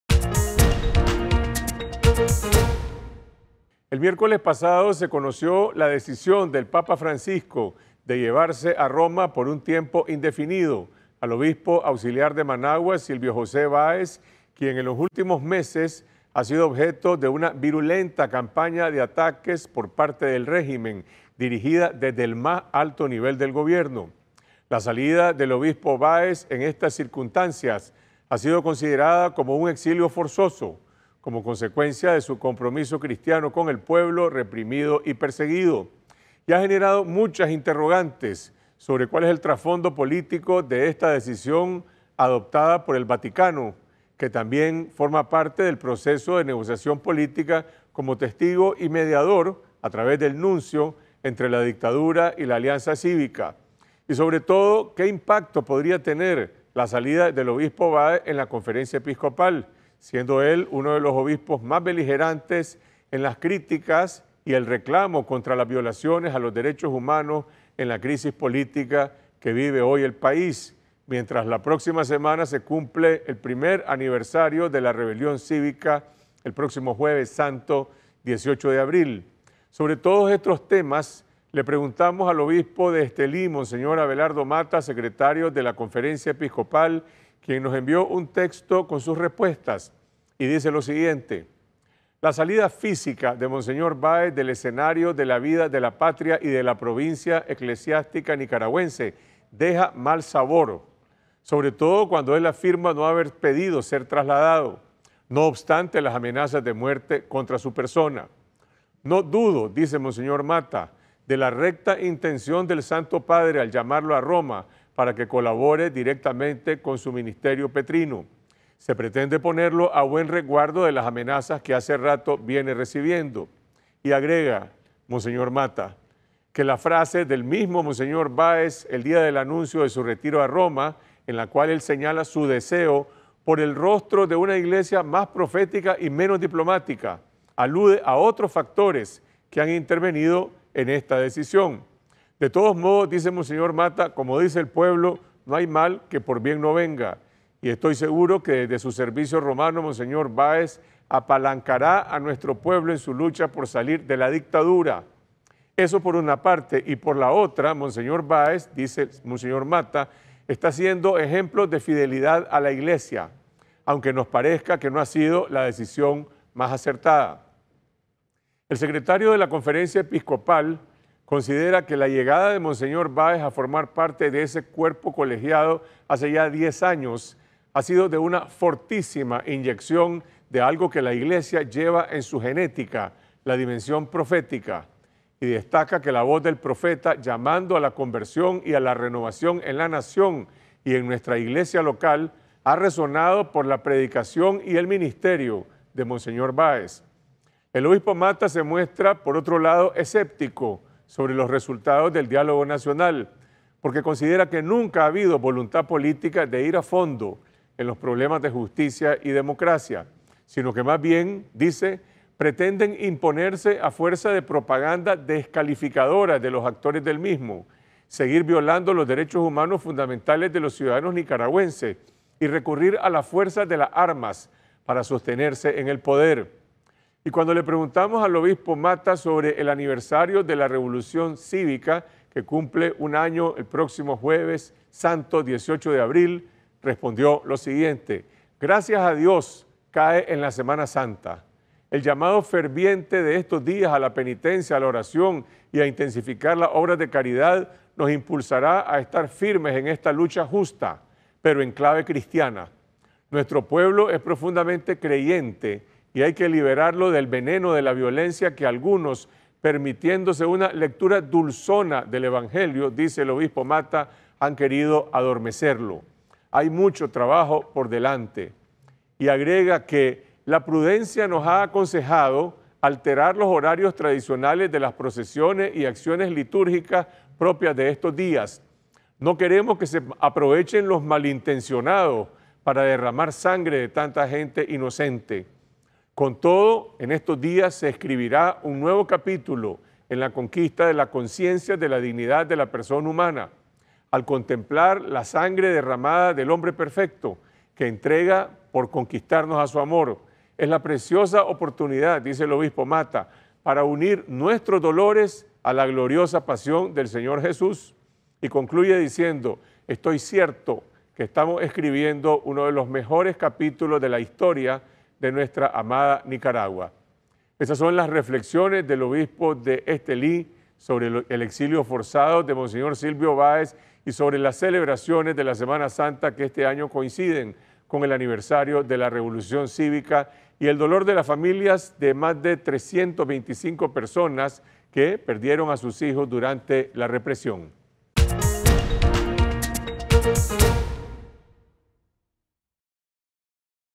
Obispo de Estelí: monseñor Báez fortalece la voz profética de la iglesia
Declaraciones de Mons. Abelardo Mata, secretario de la Conferencia Episcopal de Nicaragua, sobre la decisión del papa Francisco de trasladar a monseñor Silvio Báez a Roma.